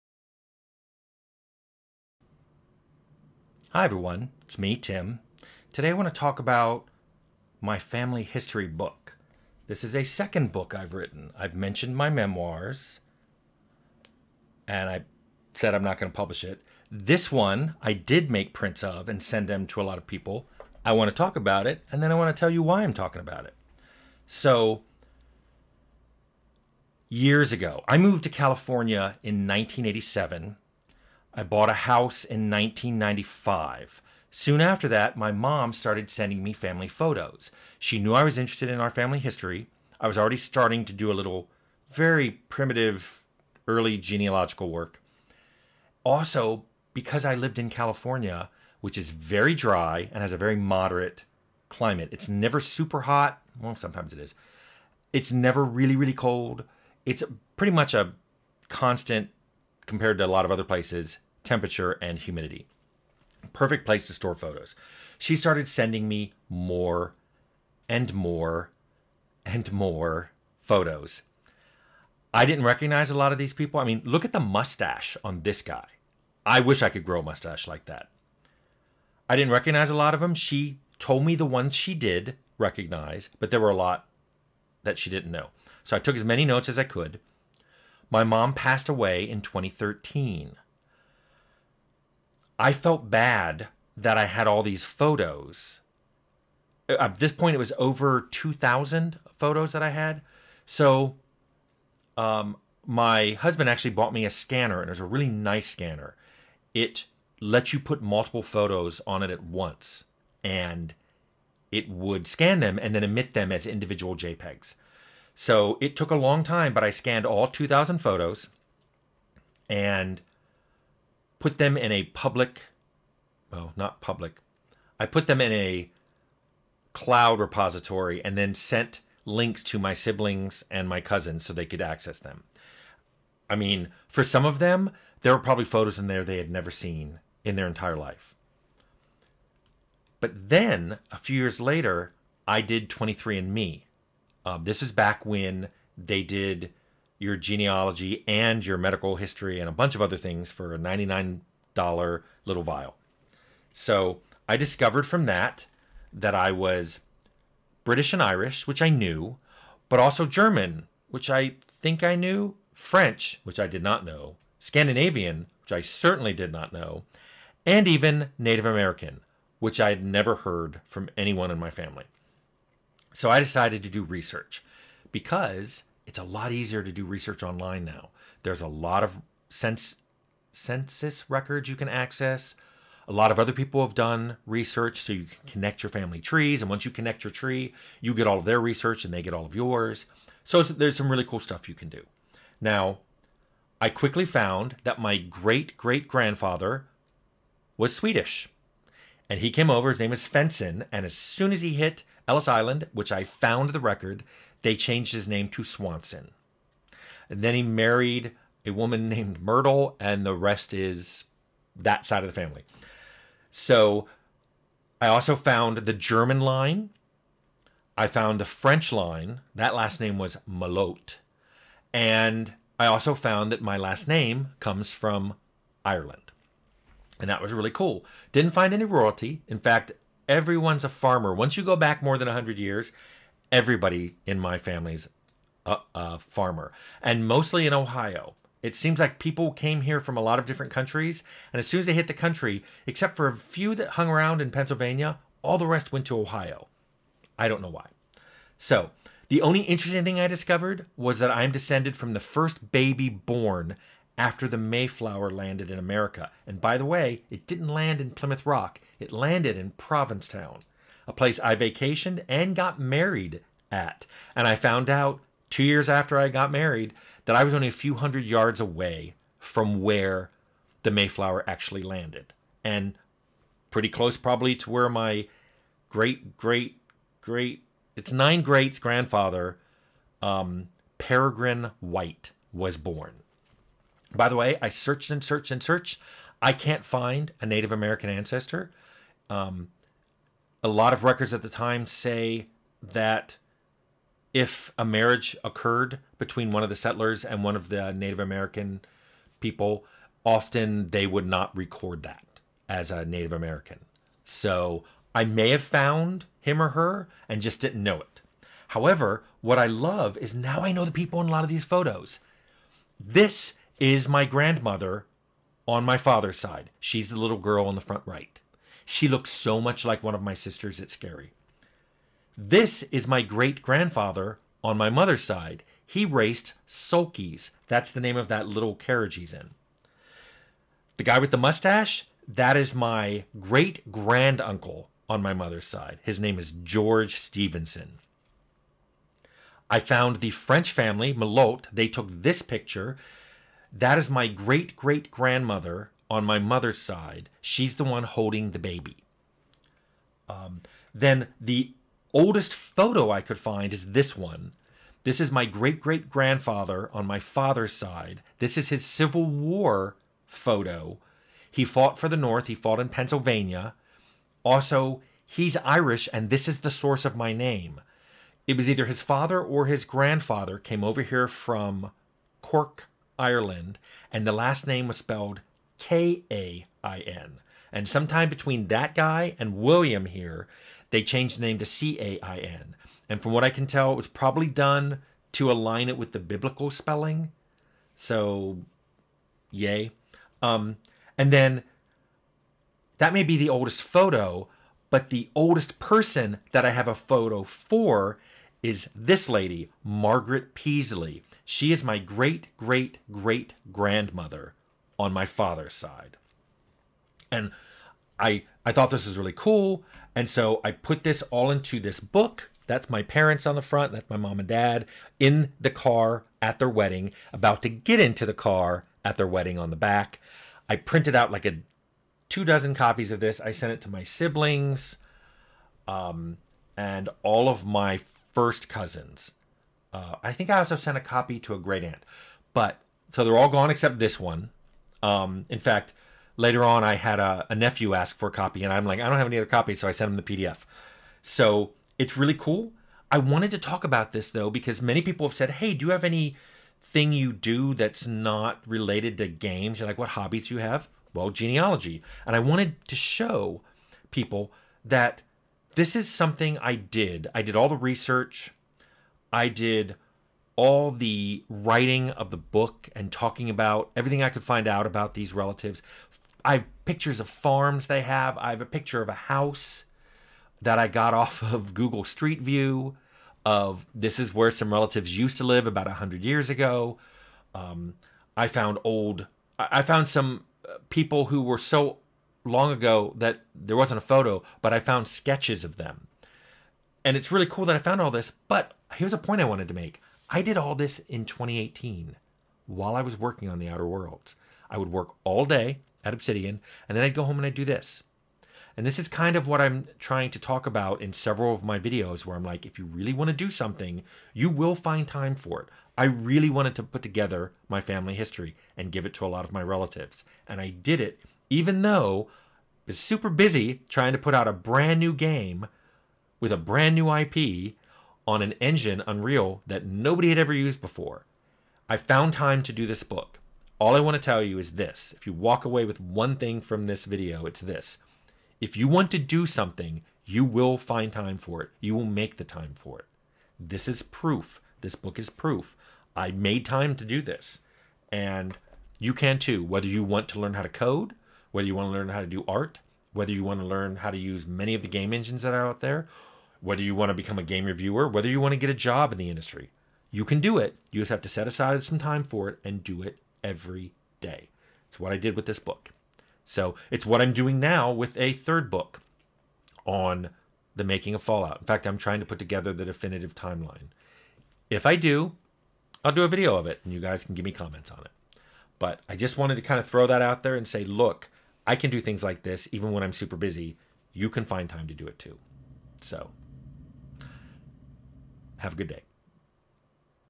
My Family History Book
I have mentioned writing my memoir, and I talk about another book I wrote about my family history. I use this story as a way to encourage viewers to find time for the things they want to do.